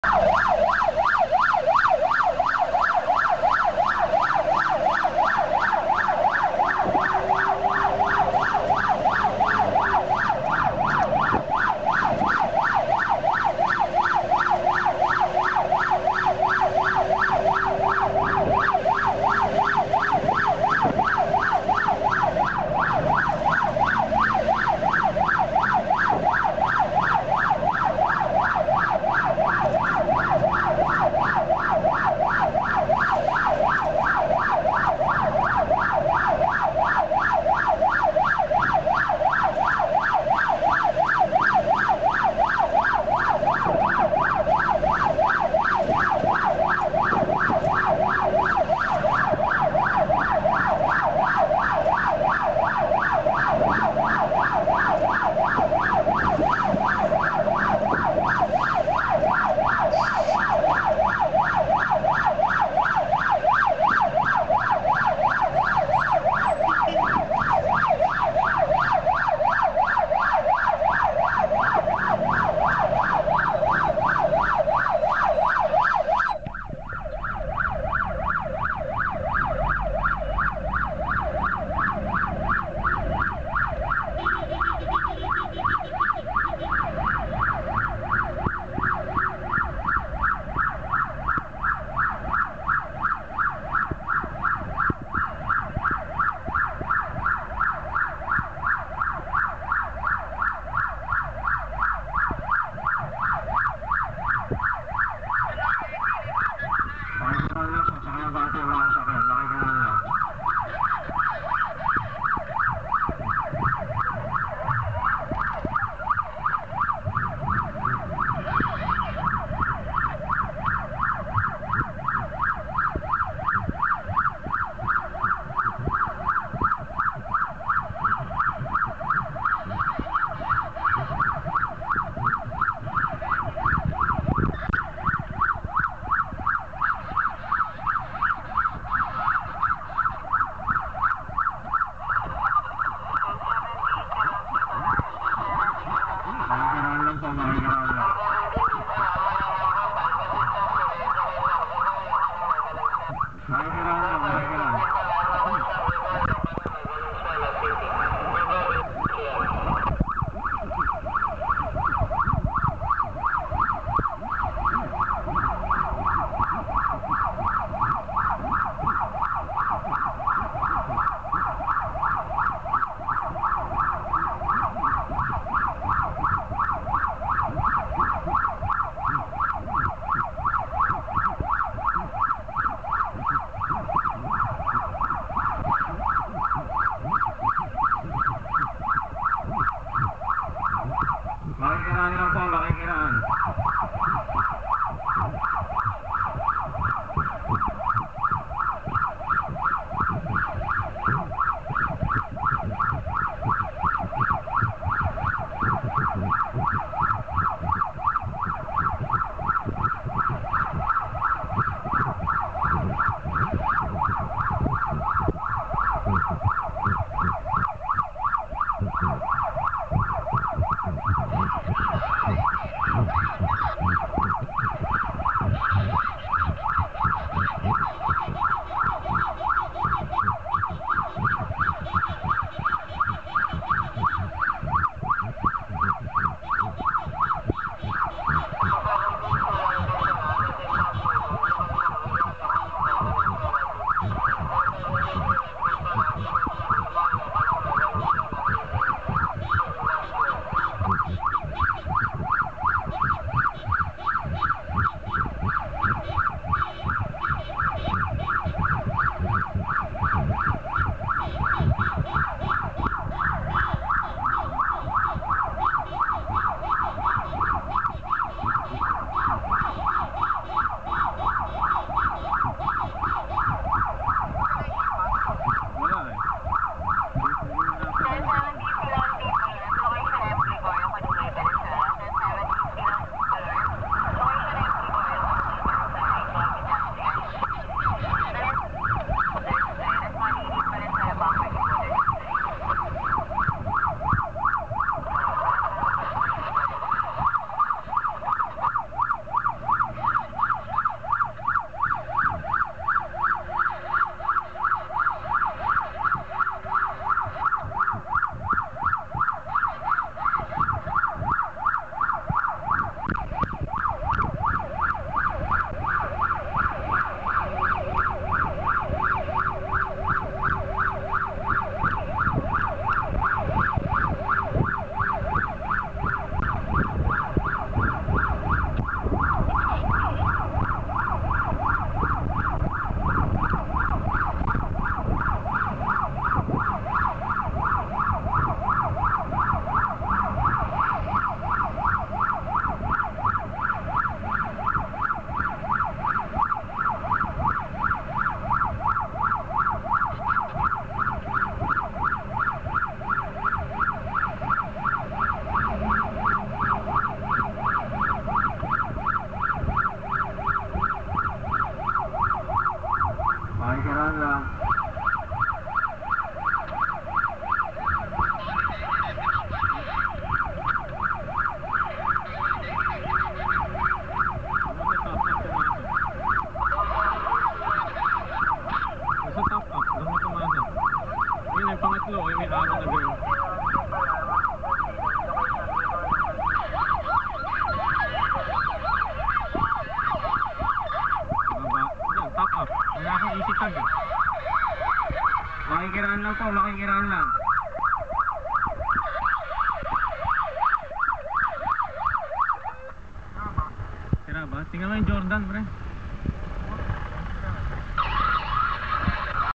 Metro Rescue Responding April 10, 2012
Responding Code 3 to a fire call in Valenzuela